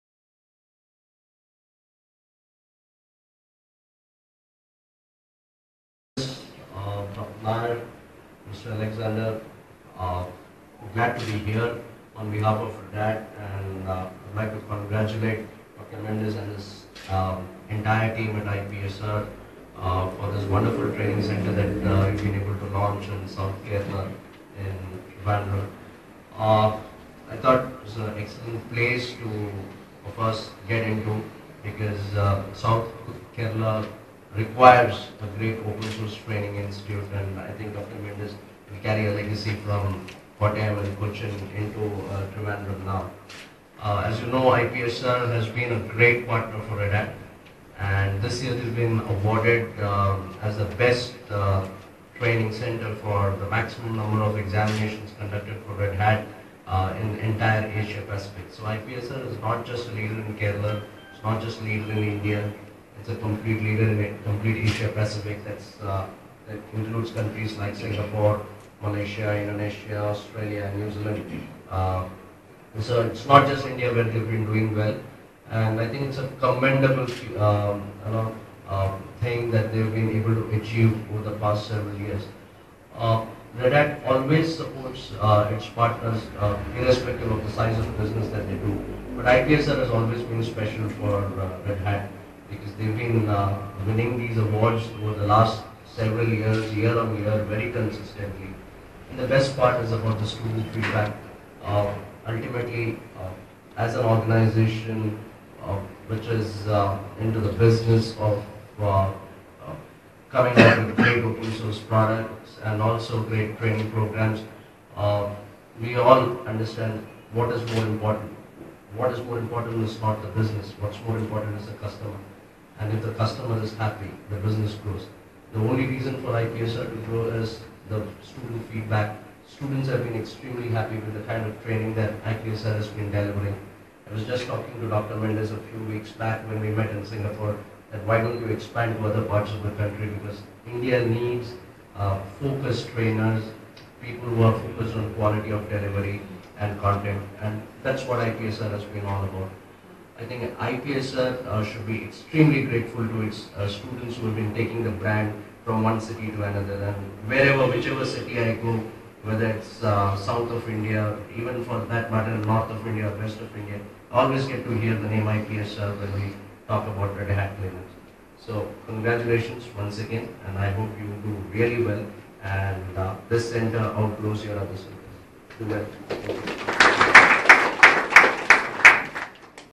Key Note Address-Mr.Sudhir Bhaskaran-Head, Global Learning Services, Red Hat India
Key Note Address by Mr.Sudhir Bhaskaran-Head, Global Learning Services, Red Hat , India